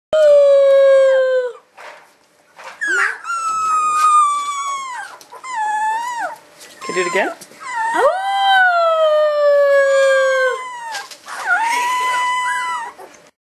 a Rhodesian Ridgeback puppy howling Tamaridge
Myah paxson Litter 2011 ..Tazor and Madison